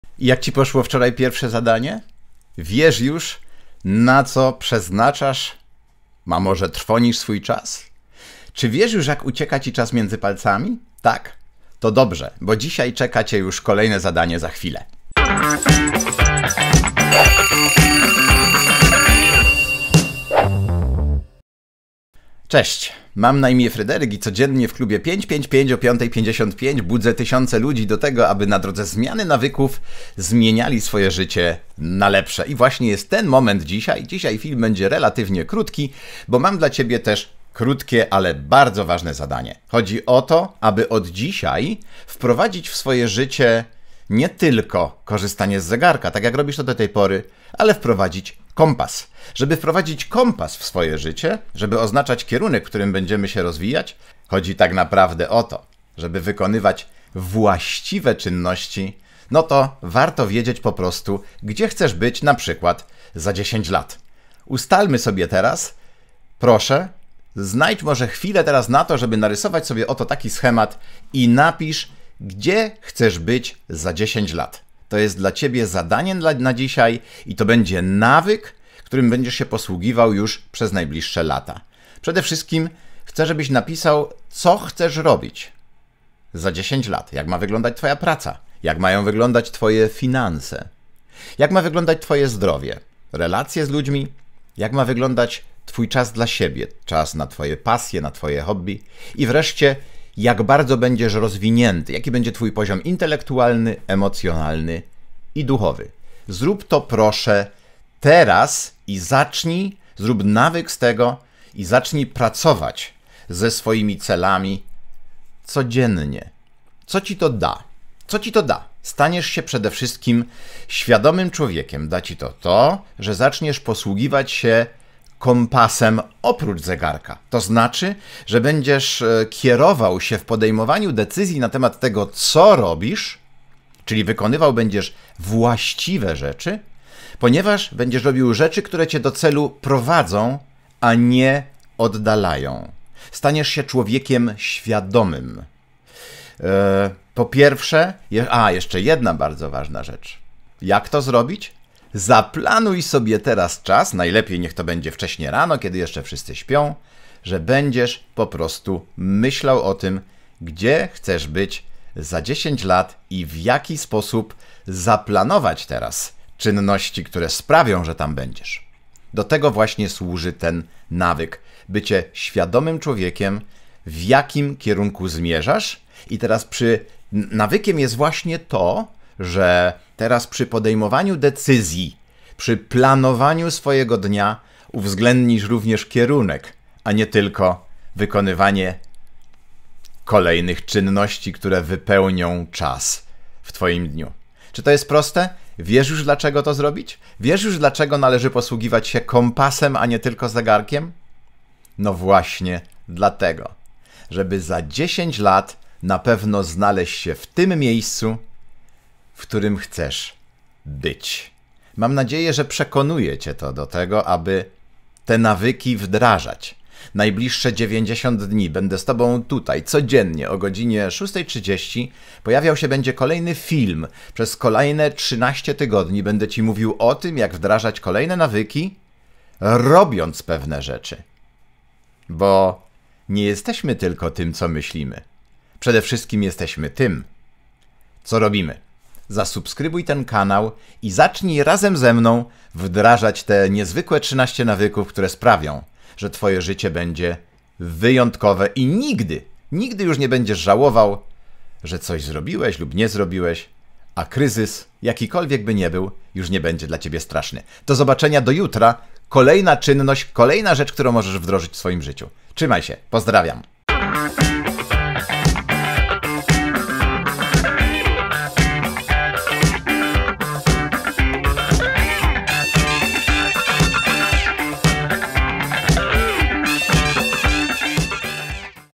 ❓ Gdzie chcesz być za 10 lat? #2/91
Gdzie chcesz być za 10 lat? Na to pytanie powinieneś znać odpowiedź obudzony i 3 nad ranem. Jeżeli nie ustaliłeś jeszcze swoich celów we wszystkich obszarach Heksagonu Szczęścia to teraz jest pora aby to zrobić.

Tydzień 1 - Zaczynaj każdy dzień z gotowym planem.